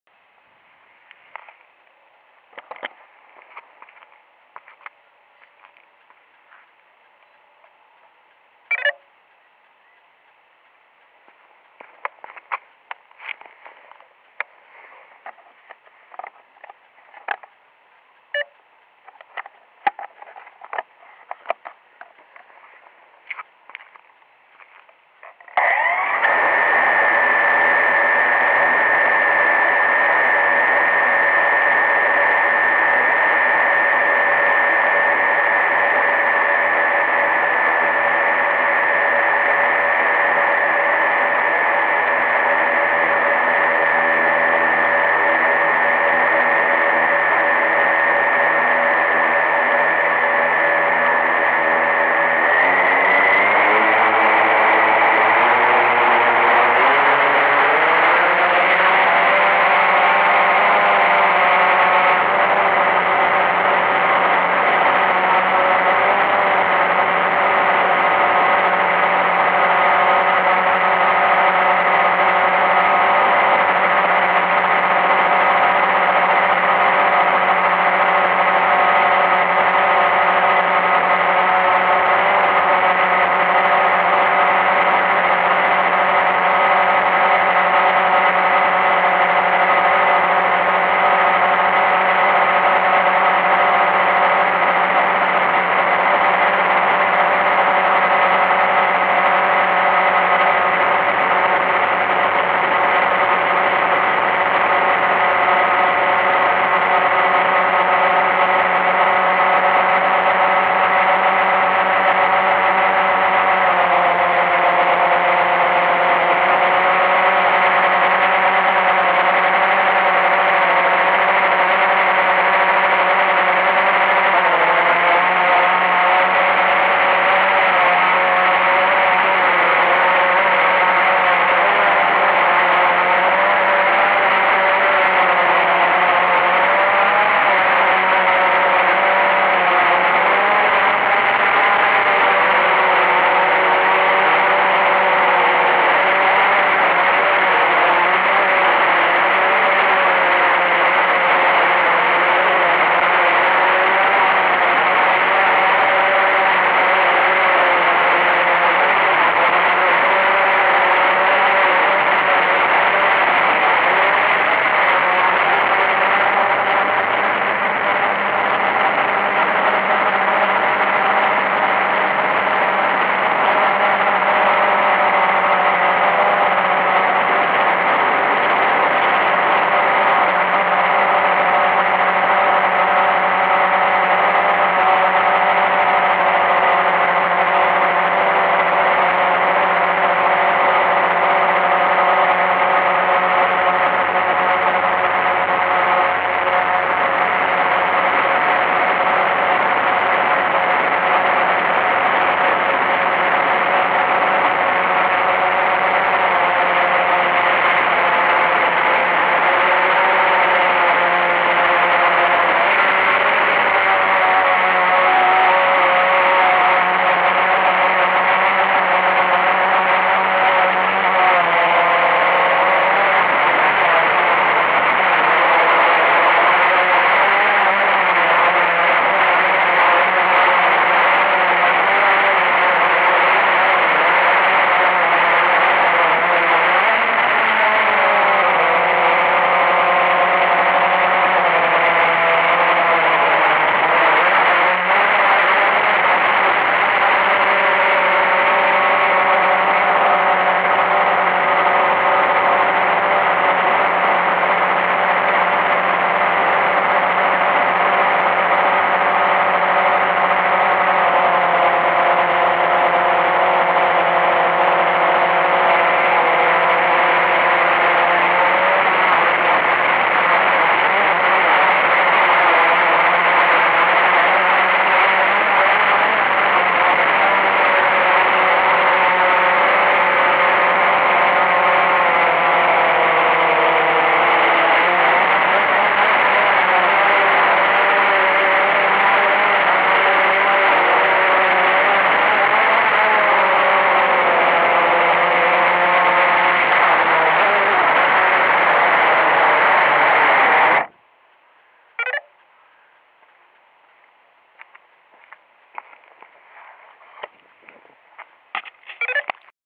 The Fog -- Nebel des Grauens Nr-2 → Wind-Turbine "Südwind S77" | Blade 350QX
Video vom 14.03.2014 | 10:10 Uhr
Melle-Gerden  / Riemsloh
Flug im Nebel und über dem Nebel bei den Südwind Windkraftanlagen S77 in Melle-Gerden (Nds)